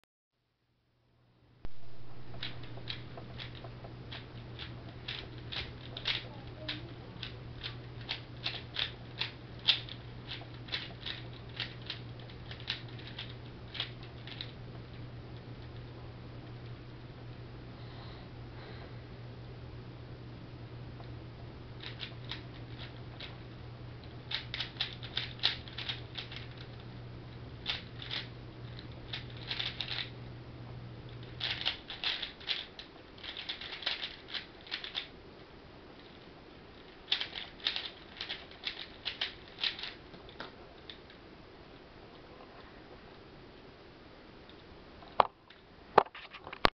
Rubiks Cube: 18.81 seconds
well it took awhile but now its here! ive gotten much faster and i plan to get faster! this cube is a dayan guhoung and its very good!